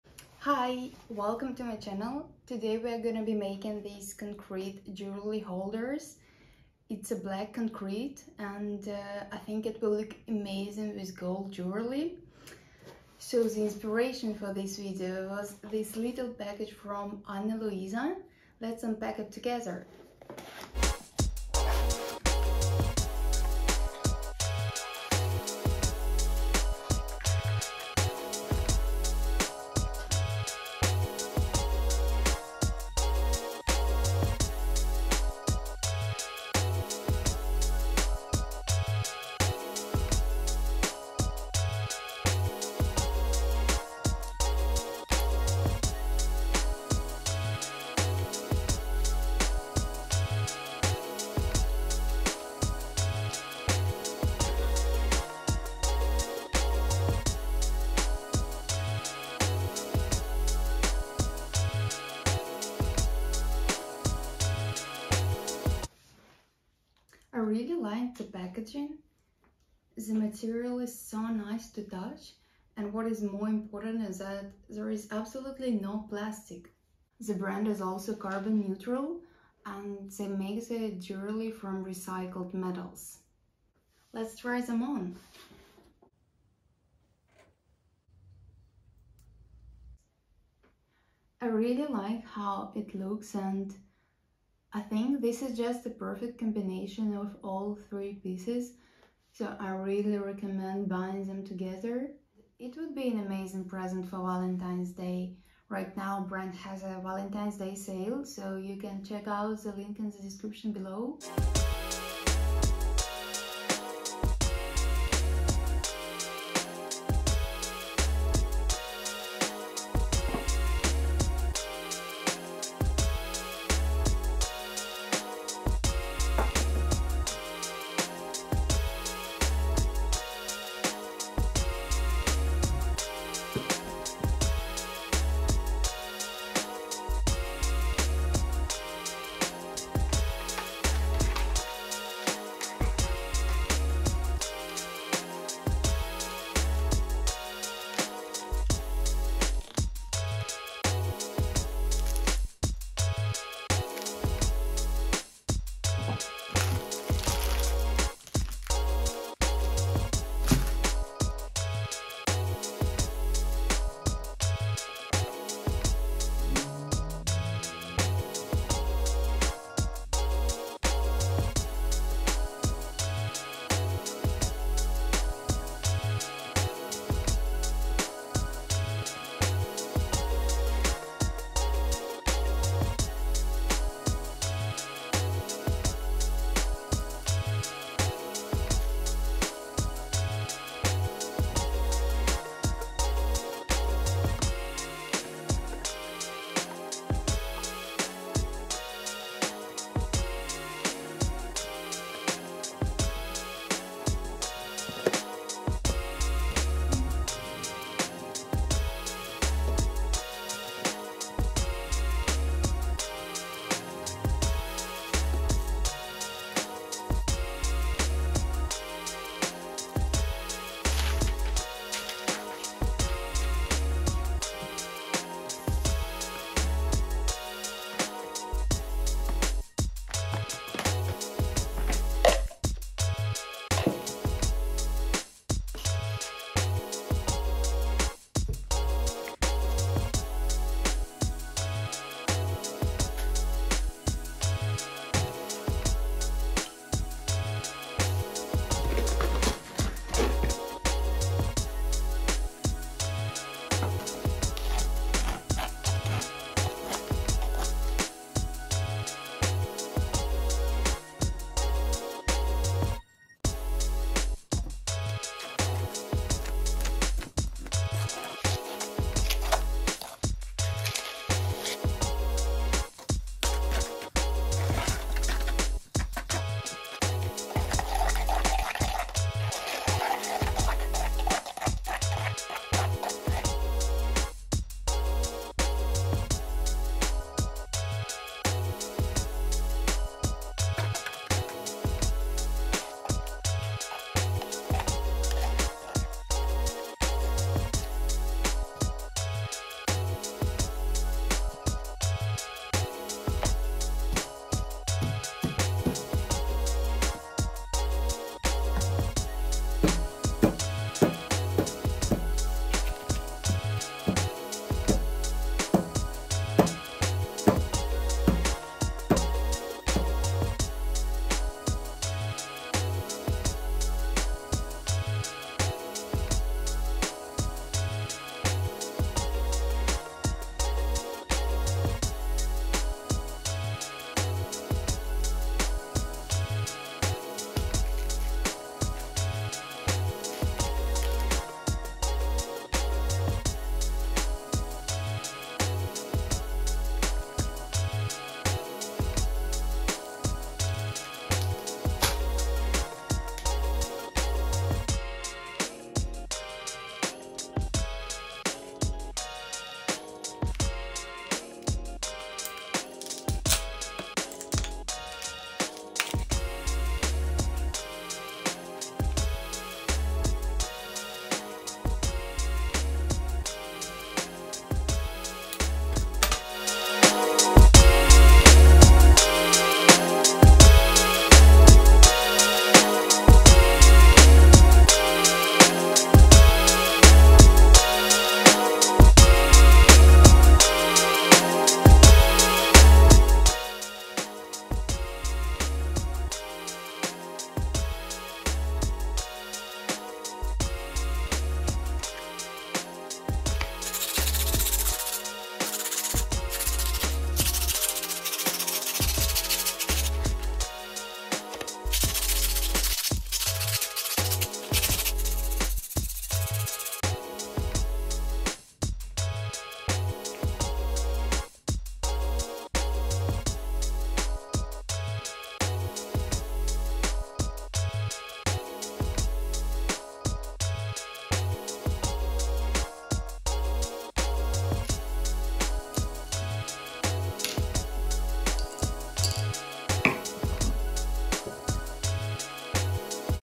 MAKING JEWELRY TRAYS | BLACK CONCRETE DIY
In this video I show how to make black concrete jewelry trays/soap trays/candle holders/coasters in a fast and easy way. I use two types of moulds: used plastic from packaging and self made moulds from plasticine.
You will need:
- cement
- sand
- plastic lids and/or plasticine
- oxide pigment in black

Time codes:
0:00 intro
1:51 final result overview
2:20 preparing moulds
3:41 mixing cement mortar